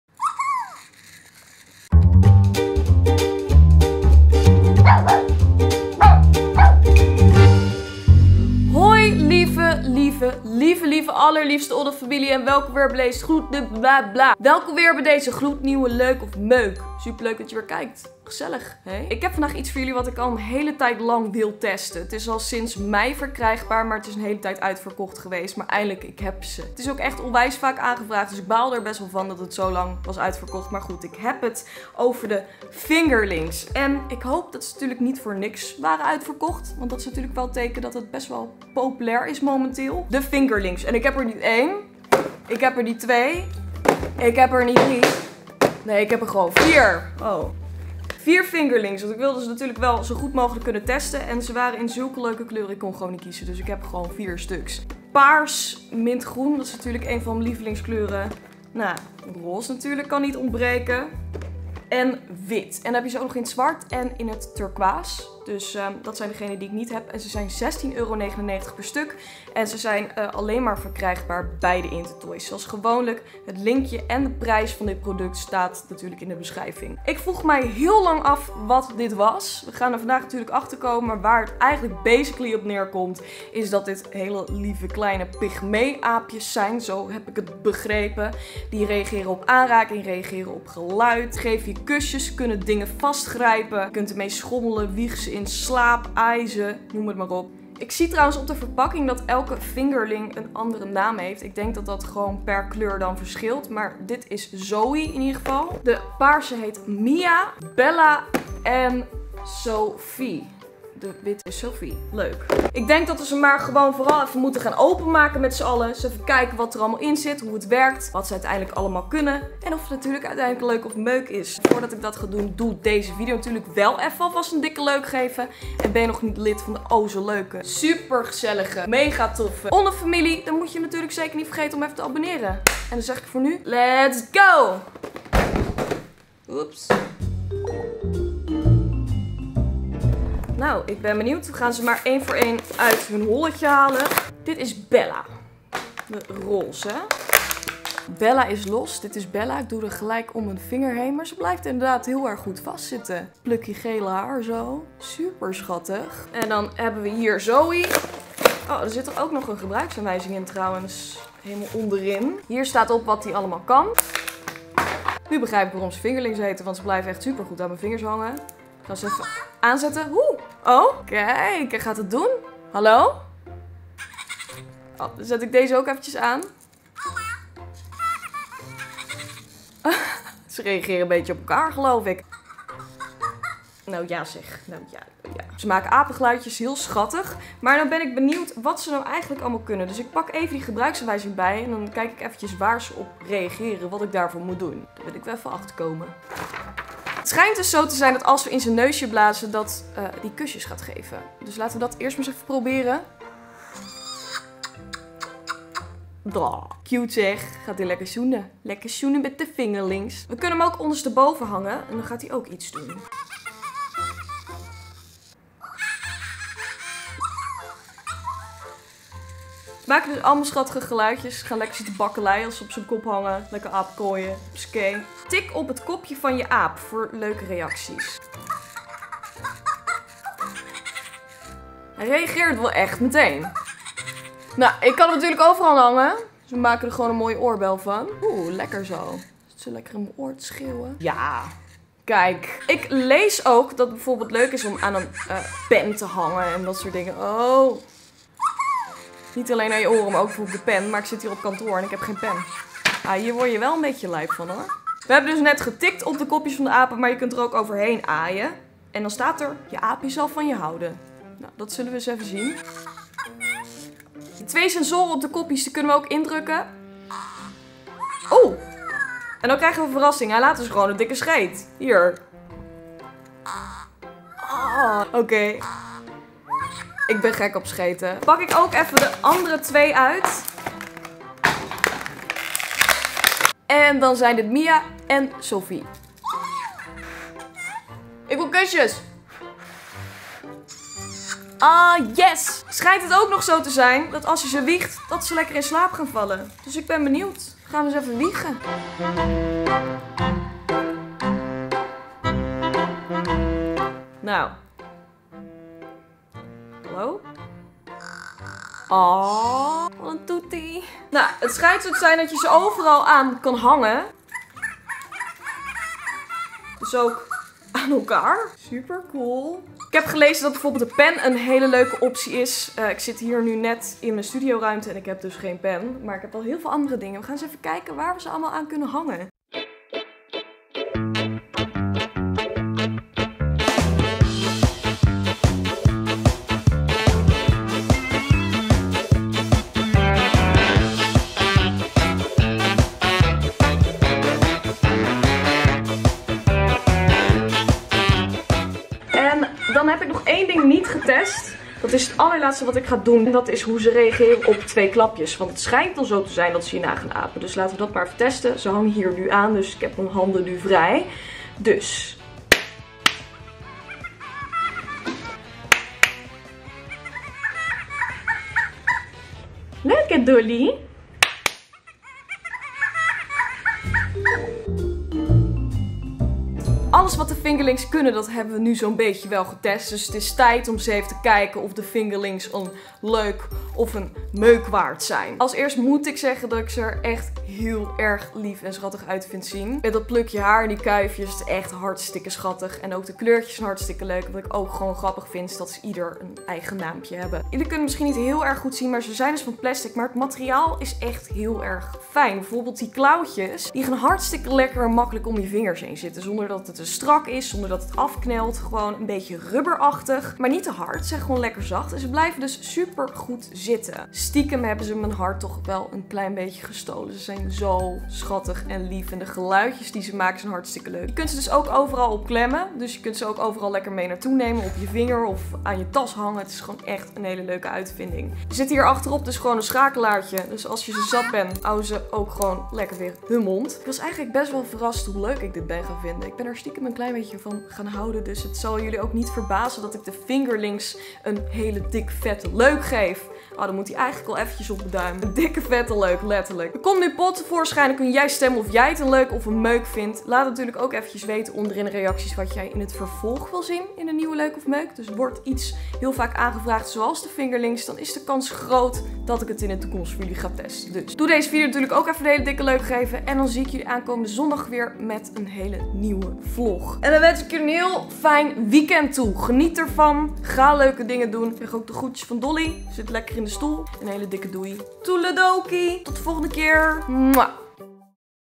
Fingerlings | LEUK OF MEUK?
Vind jij de Fingerlings ook leuk?
Oeps! In mijn video zeg ik dat de prijs €16,99 is, maar dat was een foutje. De Fingerlings zijn €19,99. Fijn weekend lieve Onnefamilie!

🌸VOLG MIJ VIA SOCIAL MEDIA🌸

✲ FANMAIL ✲
RTL MCN t.a.v. OnneDi
Nieuwezijds Voorburgwal 4-10
1012 RZ Amsterdam